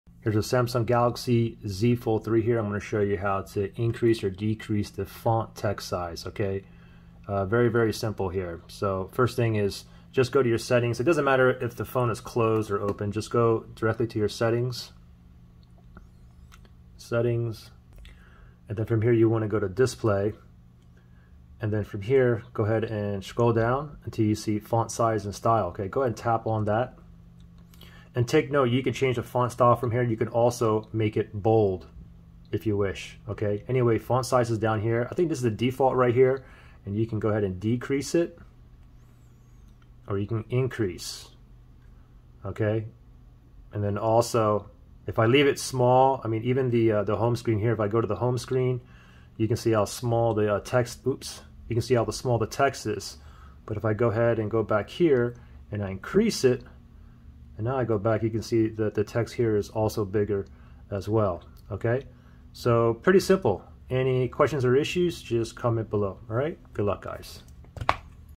Galaxy Z Fold 3: How to Change Font Text Size (Increase or Decrease)
I show how to change the font text size on the Samsung Galaxy Z Fold 3 smartphone. You will be able to make the font size bigger or smaller. Hope this helps.

---Galaxy Z Fold 3 Phone---
AU: ???